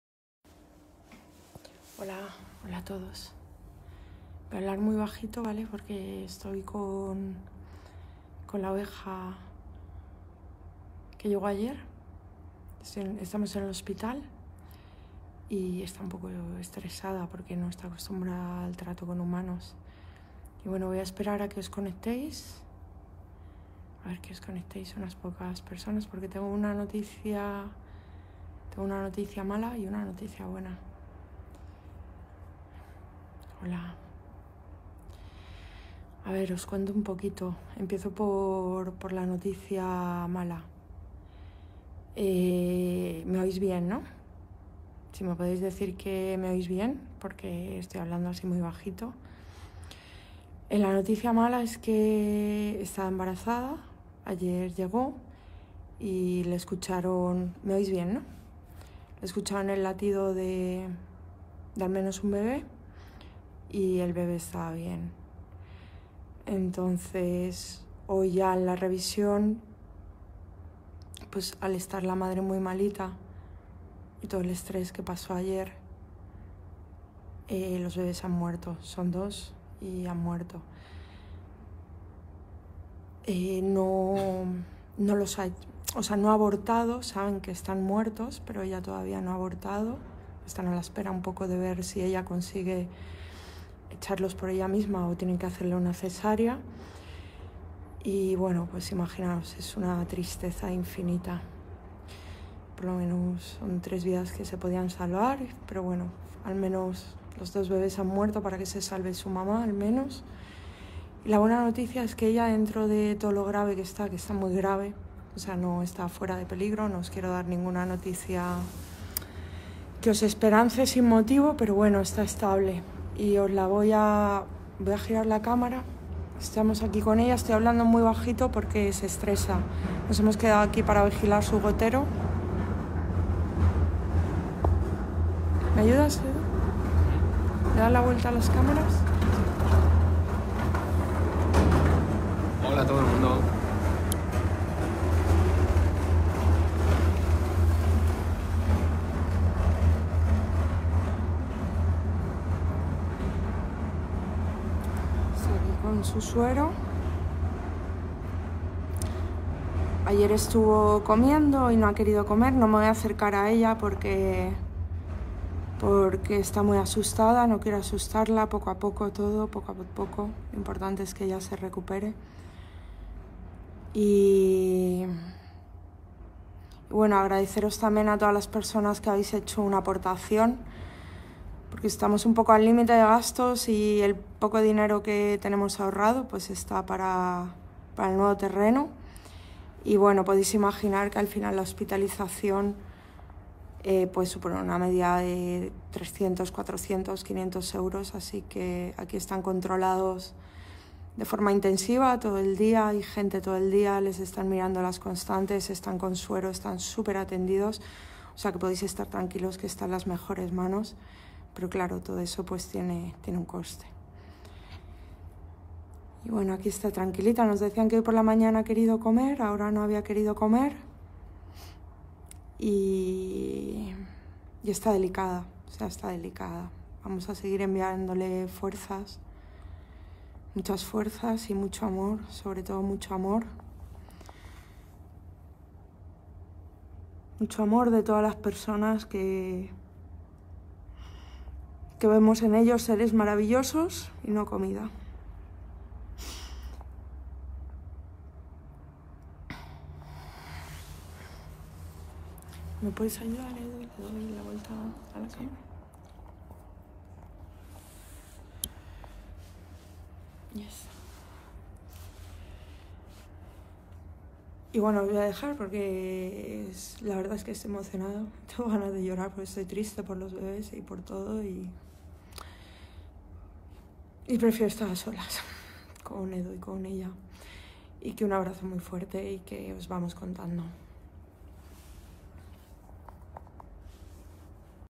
Tenemos una noticia buena y una mala sobre la oveja que rescatamos ayer muy grave.
Tiene heridas por todo el cuerpo y en la parte de atrás le falta directamente un trozo de pierna

Por favor, sabéis que estamos al límite, pero no podemos decir que no ante casos así. Ayúdanos con la campaña para que podamos salvar a más animales: